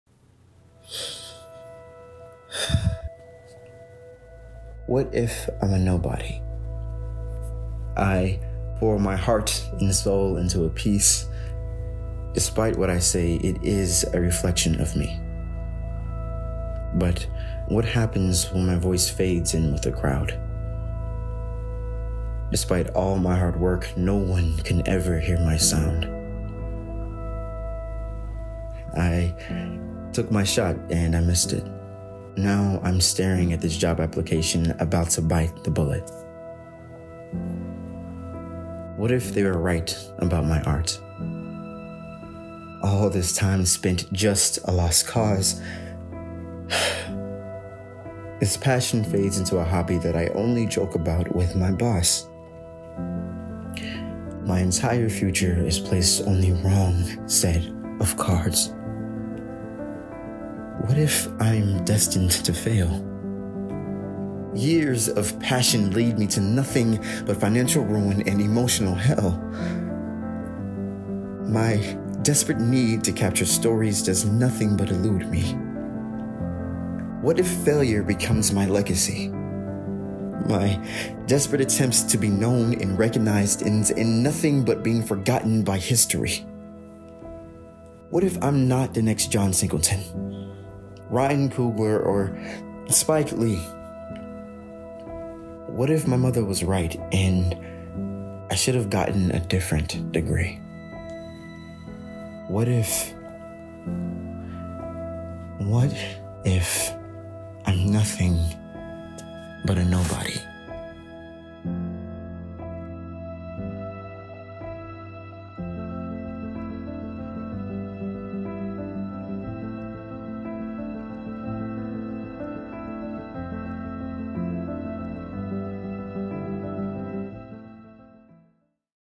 Nobody
The inner thoughts of a Film student who is weeks away from graduation. What if all of this was for nothing? What if family members were right? What if one man can't change the world? What if.